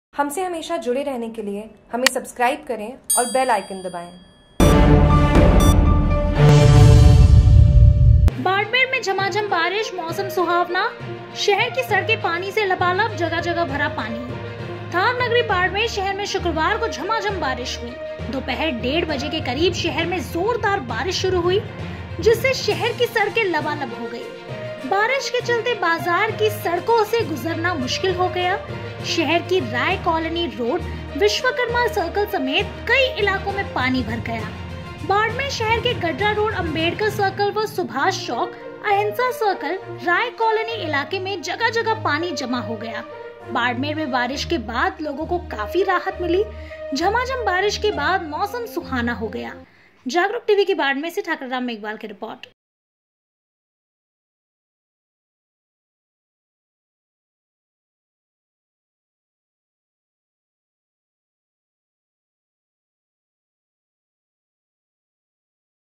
बाड़मेर में झमाझम बारिश, मौसम सुहावना हुआ जगह-जगह भरा पानी
थार नगरी बाड़मेर शहर में शुक्रवार को झमाझम बारिश हुई। दोपहर डेढ़ बजे के करीब शहर में जोरदार बारिश शुरू हुई। जिससे शहर की सड़केें लबालब हो गईं। बारिश के चलते बाजार की सड़कों से गुजरना मुश्किल हो गया। शहर की राय कॉलोनी रोड, विश्वकर्मा सर्किल समेत कई इलाकों में पानी भर गया। बाड़मेर शहर के गडरा रोड अम्बेडकर सर्किल व सुभाष चौक, अहिंसा सर्किल, राय कॉलोनी इलाके में जगह-जगह पानी जमा हो गया। बाड़मेर में बारिश के बाद लोगों को काफी राहत मिली। झमाझम बारिश के बाद मौसम सुहाना हो गया।